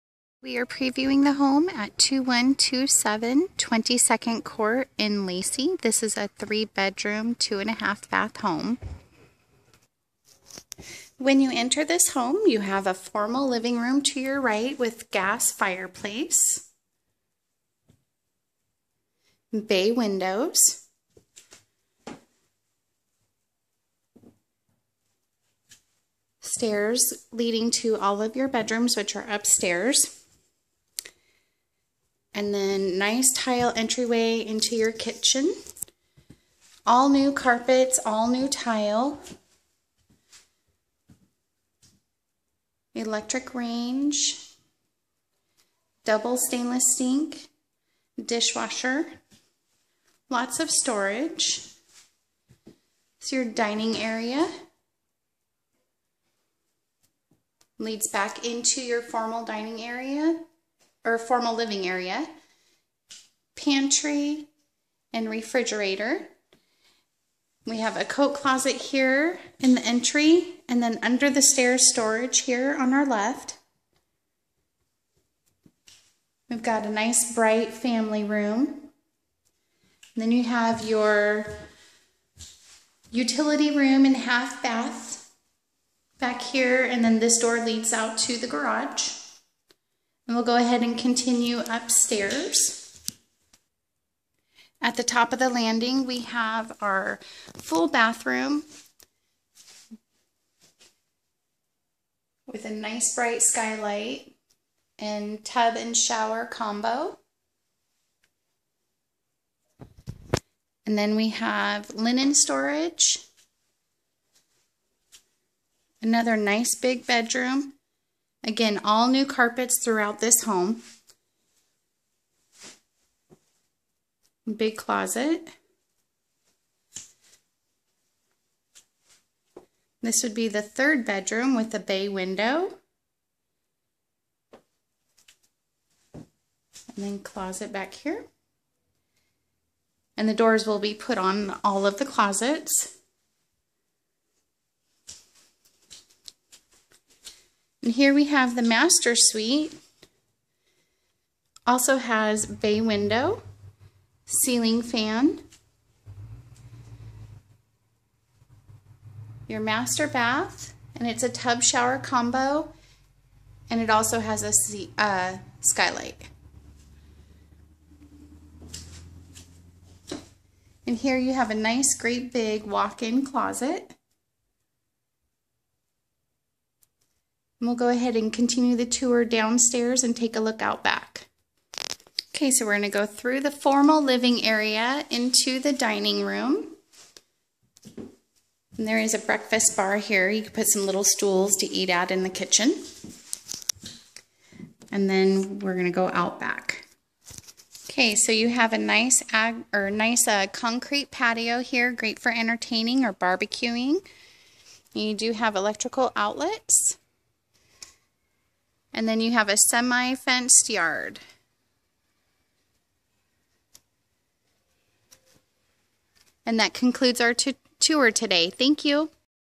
Rented! 3 bed/2.5 baths - 2127 22nd Ct SE Lacey, WA 98503
New Carpet and tile! 3 bedrooms and 2.5 bath, 2 story home in Shady Wood sub-division. Living room, dining area off kitchen, family room. Kitchen with breakfast bar, refrigerator, dishwasher, stove/range. Forced air, gas fireplace. 2 car garage w/openers. Washer/dryer hook-ups. Large lot w/patio on a quiet setting cul-de-sac. 1617 sq ft. North Thurston School District: E:Lacey M:Chinook H:North Thurston. City water and sewer. One Small Pet Negotiable upon approval and non refundable pet deposit.